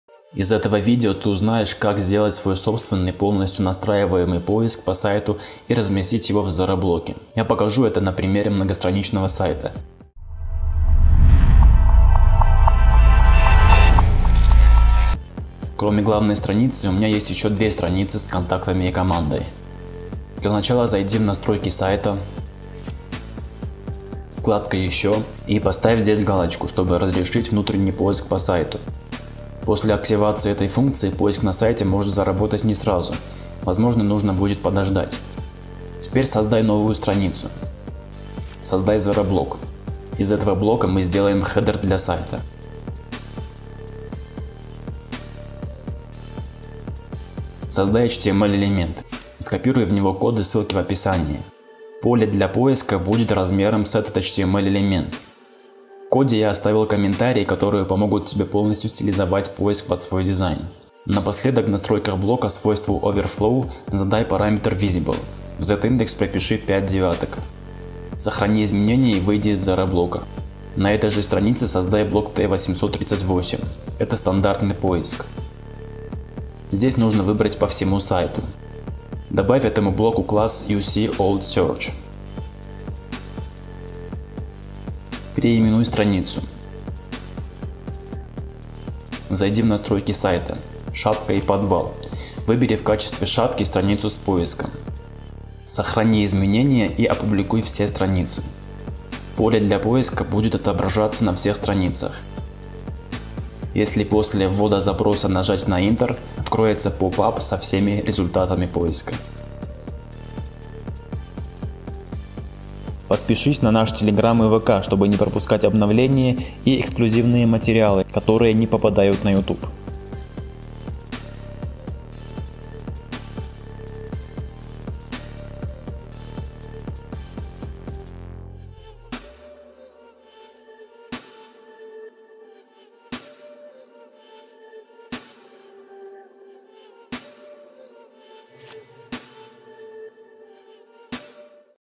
Свой поиск по сайту в Tilda Zero block
Как сделать свой полносью настраиваемый поиск по тильда сайту в zero block? Посмотри это видео до конца)